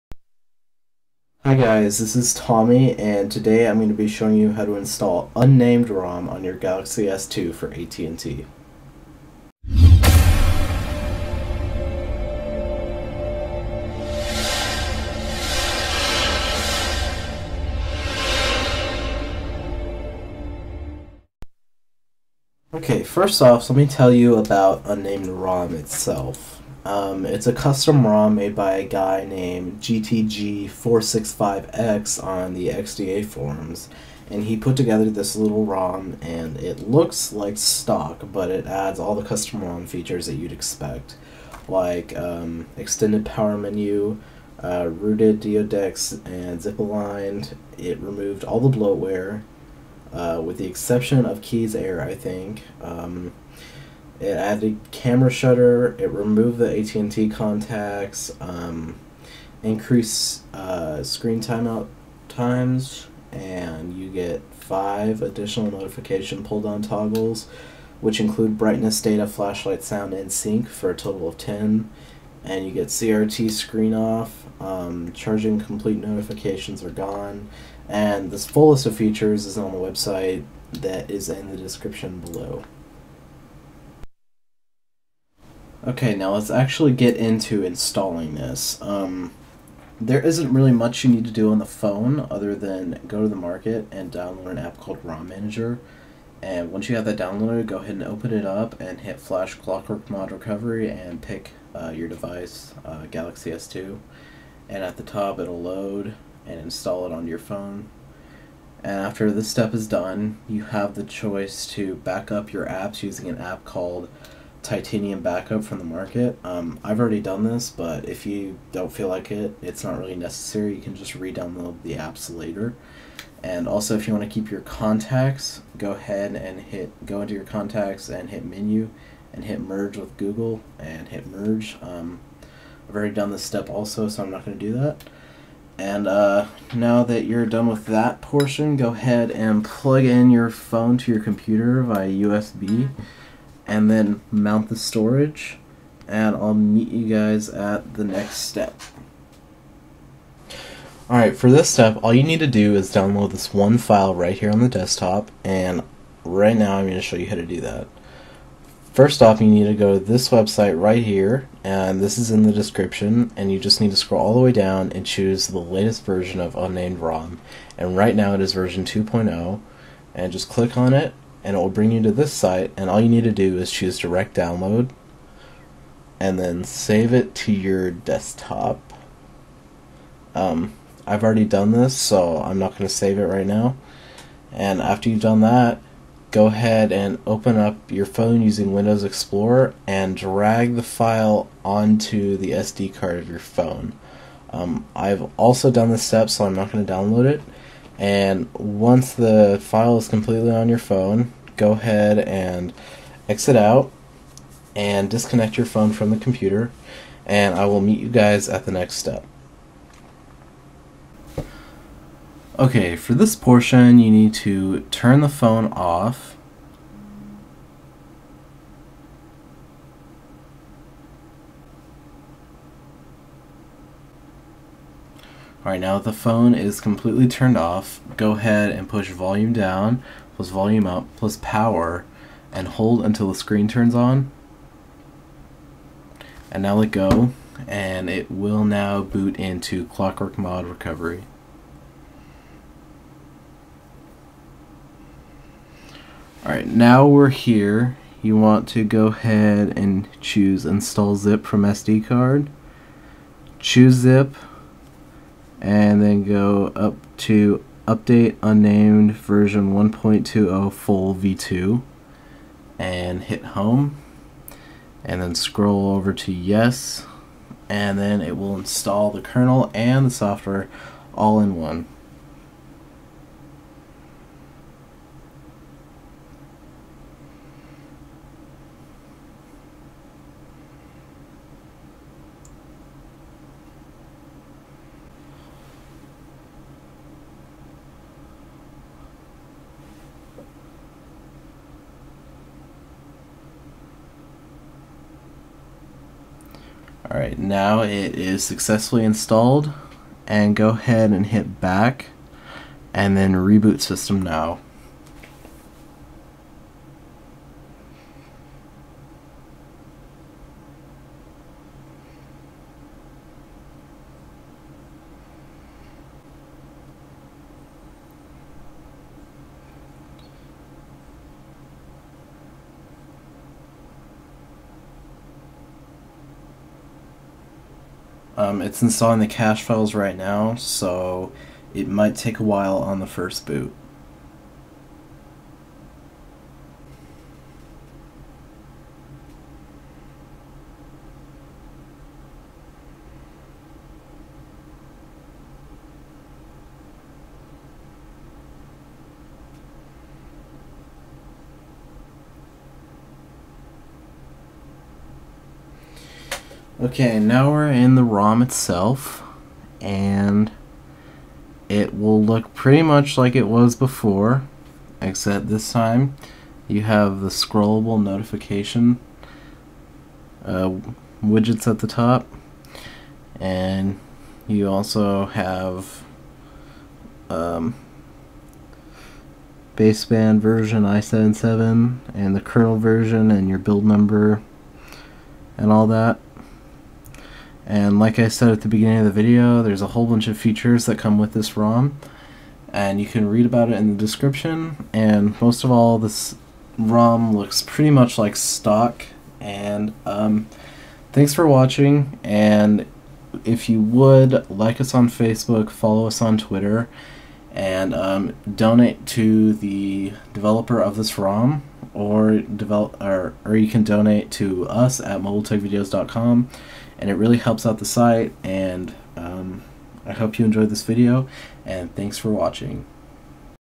Samsung Galaxy S II AT&T Edition (How To Install UnNamed ROM)
Tommy teaches you just how easy is to install UnNamed ROM for your AT&T Galaxy S II in this video.